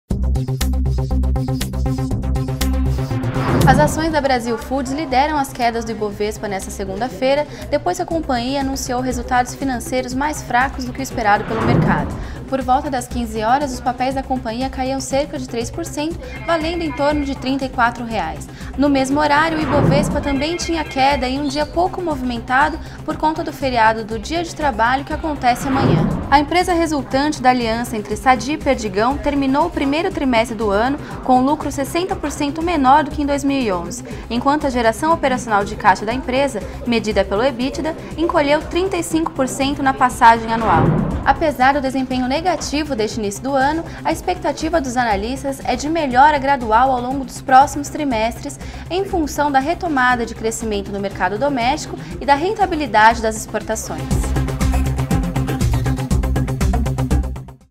InfoMoney Agora: ações da Brasil Foods têm queda após balanço abaixo do esperado
30 de abril de 2012 • 15h25
Por: Mariana Mandrote

O InfoMoney Agora da tarde desta segunda-feira (30) traz informações sobre o desempenho das ações  da Brasil Foods (BRFS3), empresa resultante da aliança entre Sadia e Perdigão. Os papéis da companhia têm queda após o balanço do primeiro trimestre desagradar o mercado.